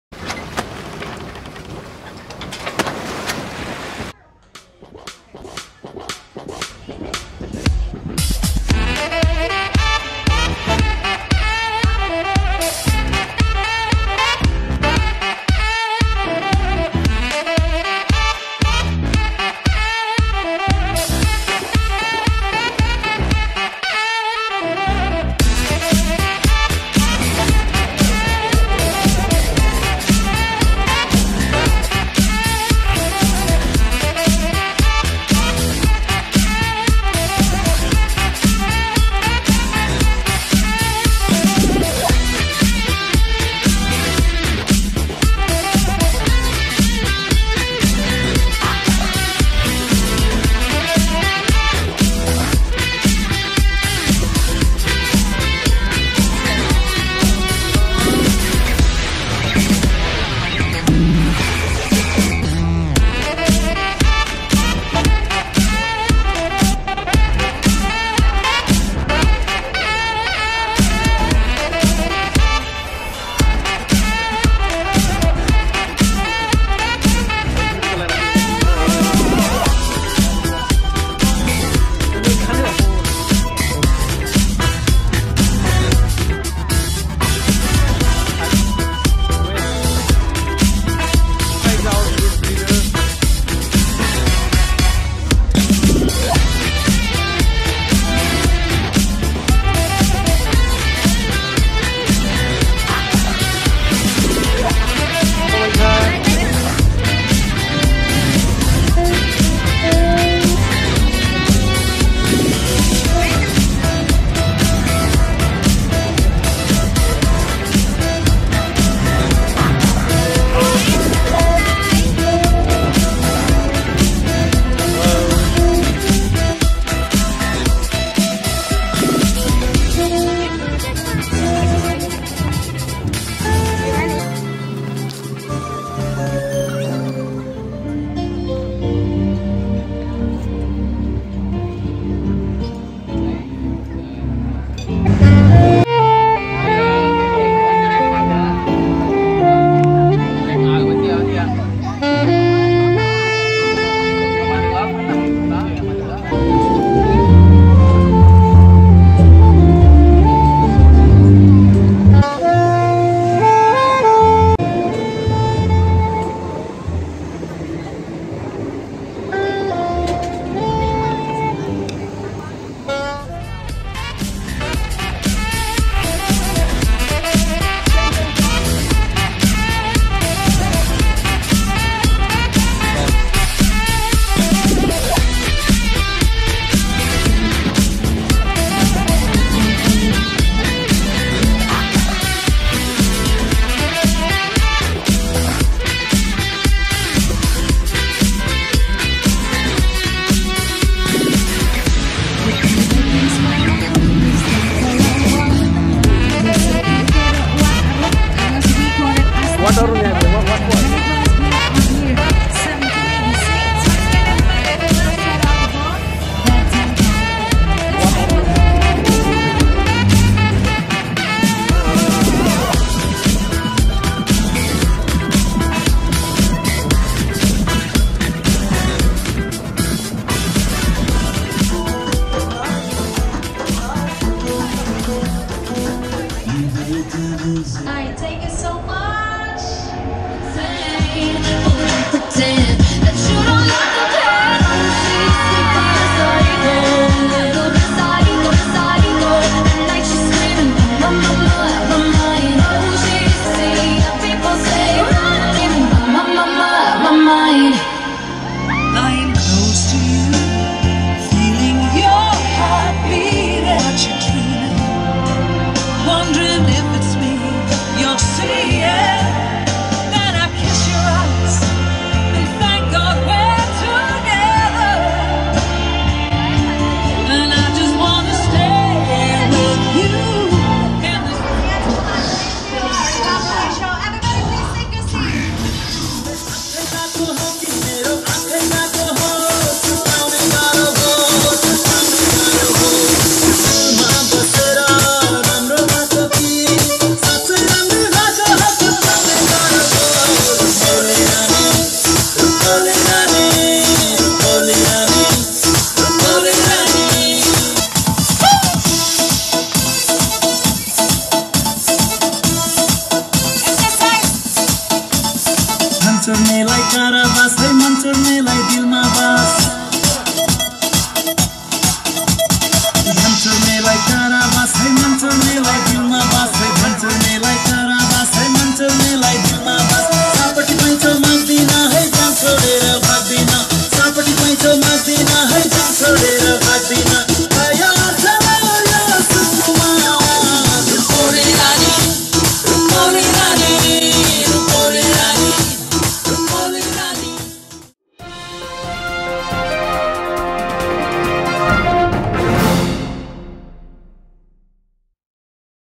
Cruise dinner in Bangkok
Having Cruise dinner with my family in Chao Phraya river, Bangkok was amazing. Unlimited food... unlimited fun unlimited enjoyment. wow there is no words to express.June 2019 ChaoPhraya # Cruisedinner